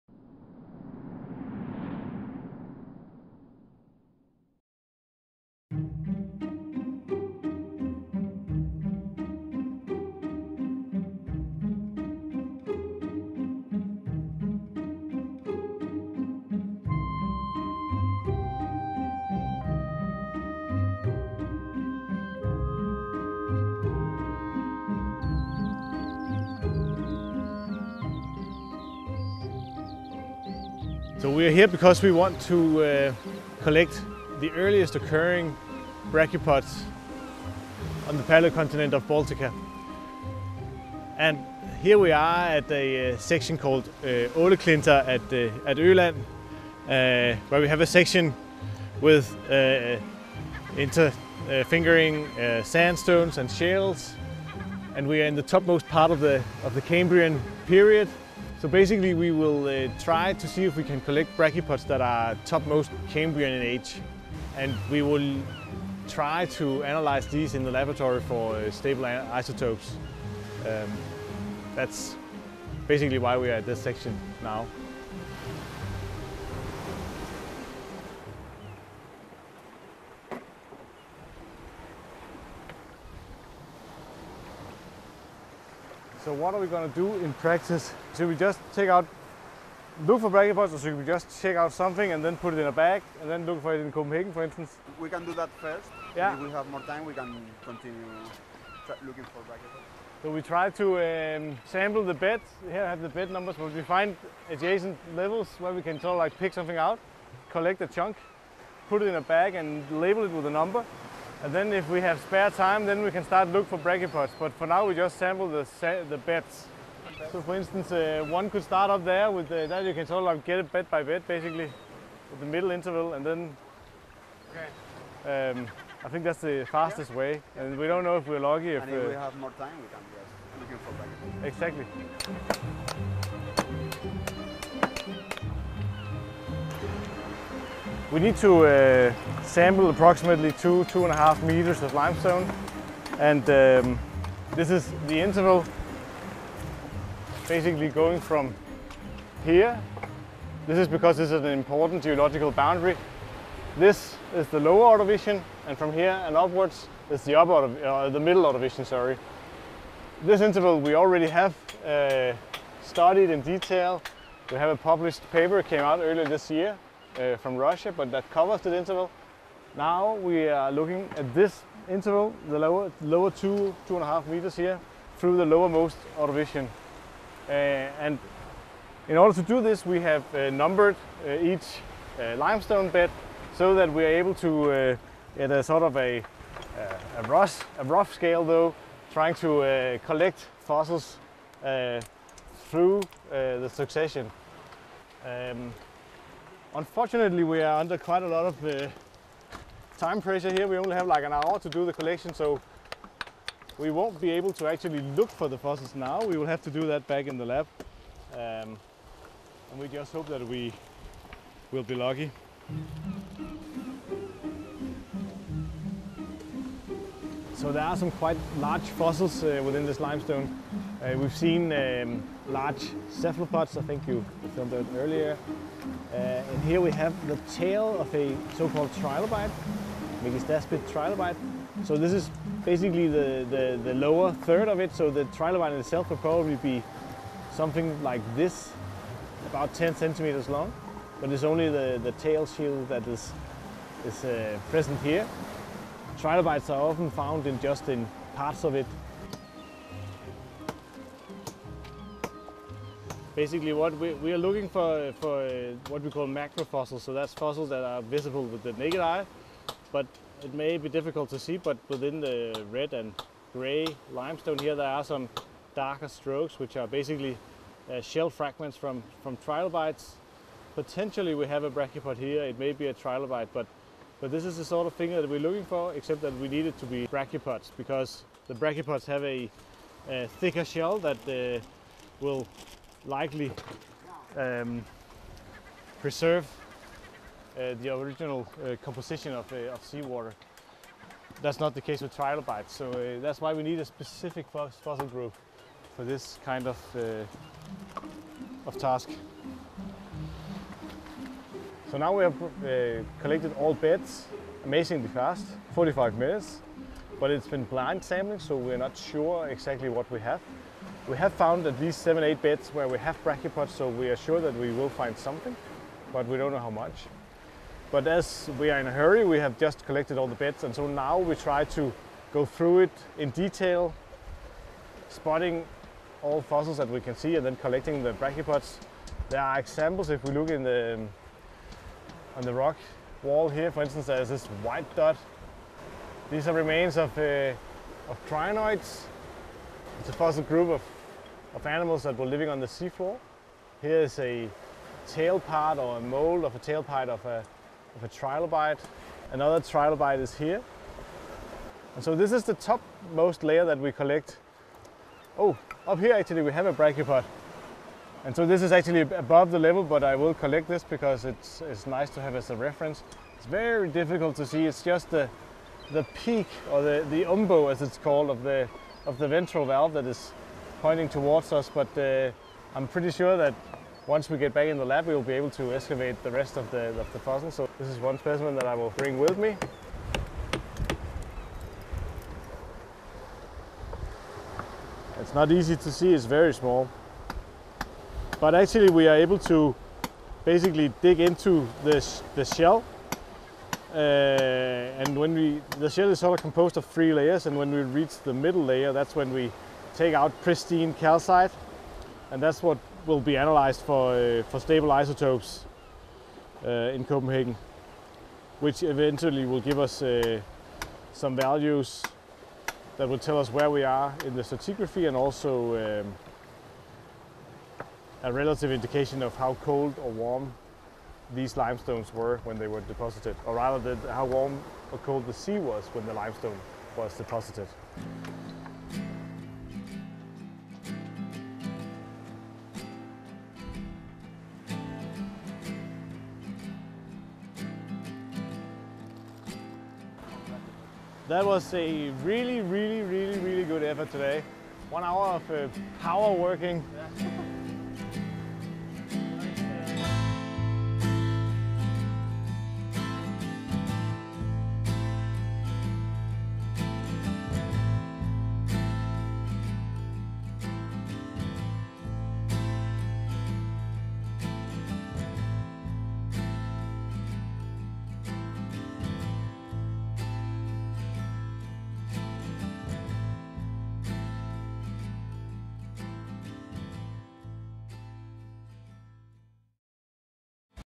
The Ordovician climate paradox – searching for earliest evidence of an ice age
Long believed to have been an integral part of an Early Palaeozoic super-greenhouse, Ordovician climate has in recent years undergone a remarkable revision. Thus, it now appears that a much cooler climate prevailed throughout most of the period. Here, a team of palaeontologists set out to search for the earliest fossil and geochemical evidence for an Ordovician icehouse on the Swedish island of Öland.